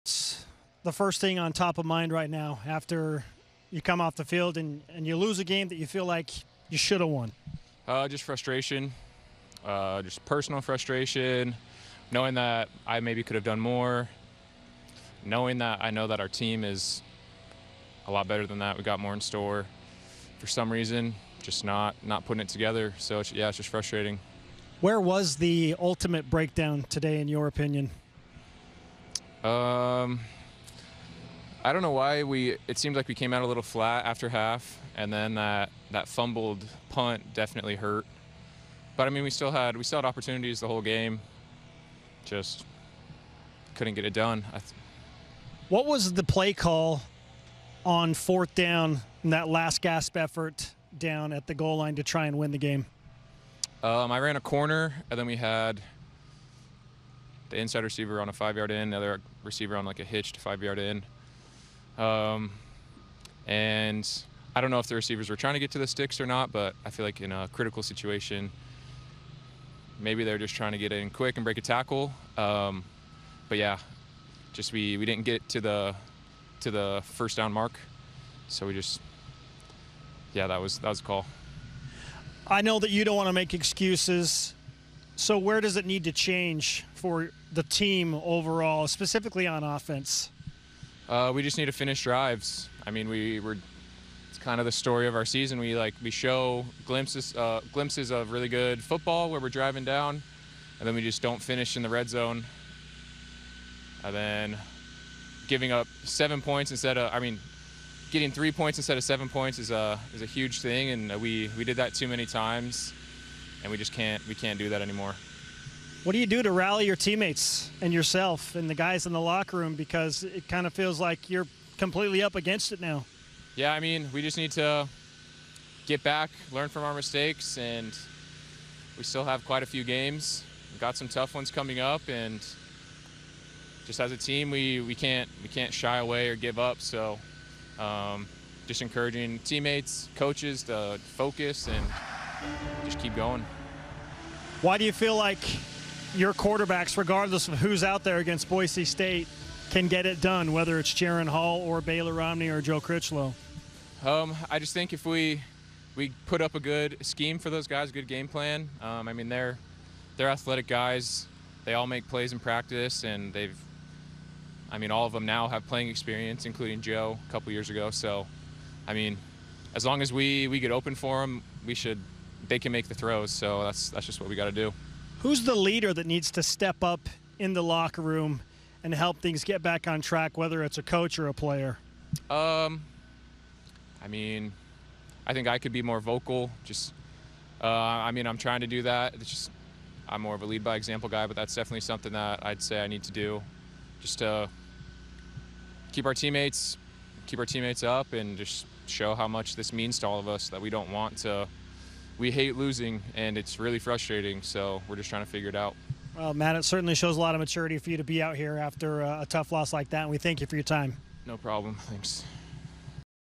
Matt Bushman Postgame Interview at USF
Matt Bushman joins Spencer Linton to talk about needed improvements for the BYU football team from Tampa, Florida.